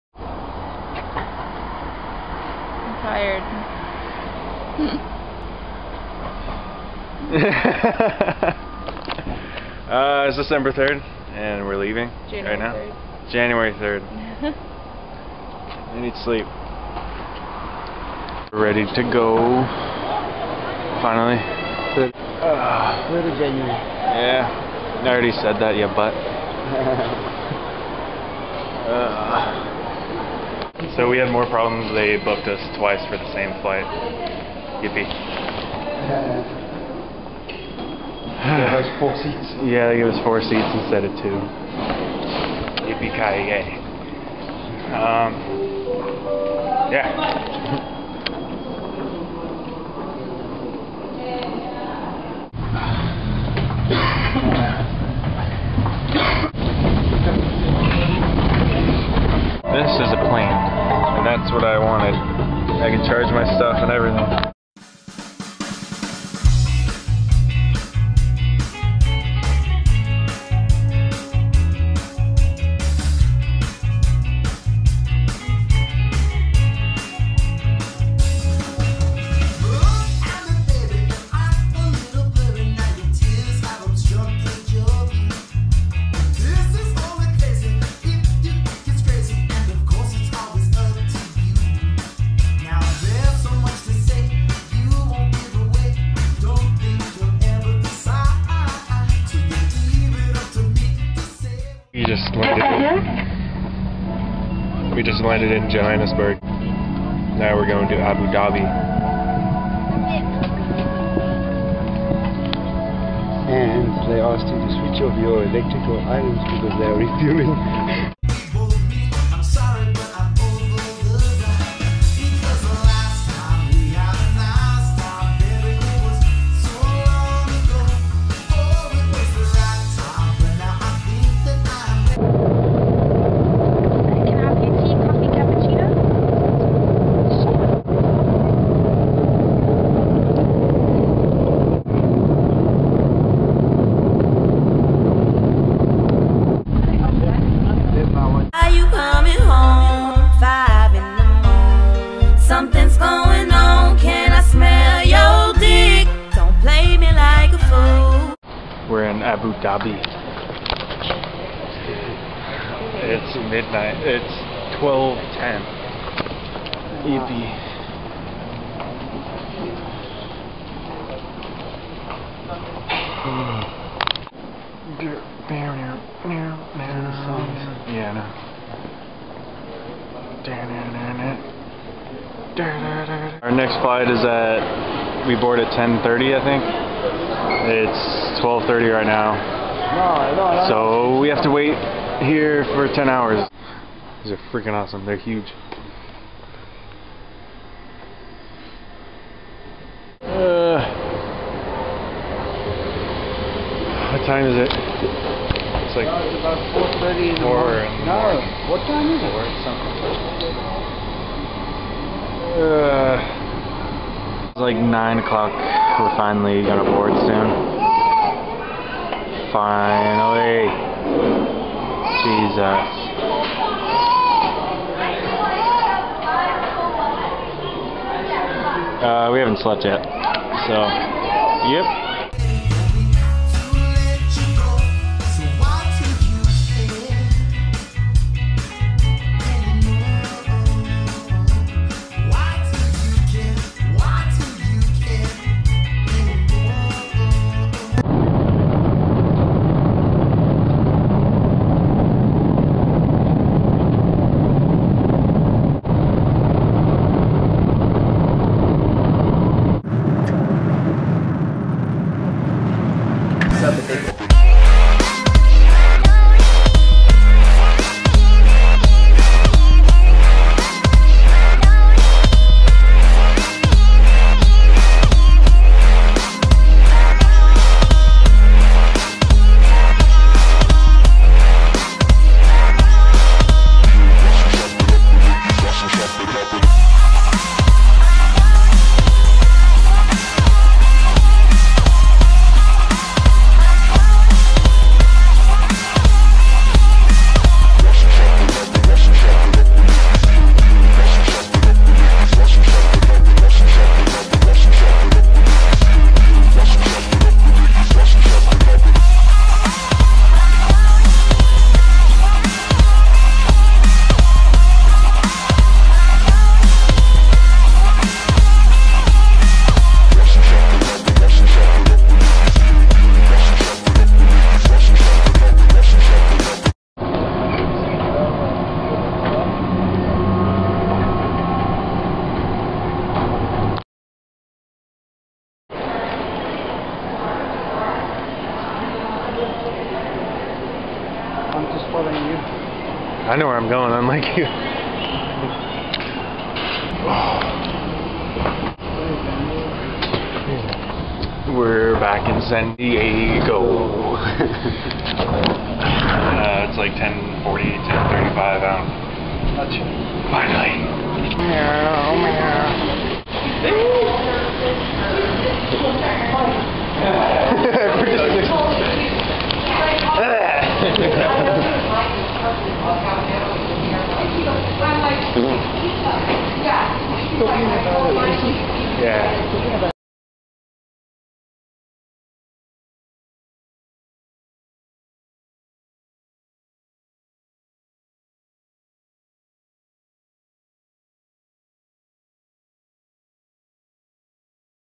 DAYS 31 & 32 - 1/3 & 1/4/2012 FLYING HOME!
Flying home from Cape Town, South Africa. Went from Cape Town to Johannesburg, Johannesburg to Abu Dhabi, Abu Dhabi to Chicago, Chicago to San Diego. Left on 1/3 at 10 AM got home on 1/4 at 10:20 PM. Had to edit the music in this vlog so the audio wouldn't get muted by YouTube again because of copyright bullshit..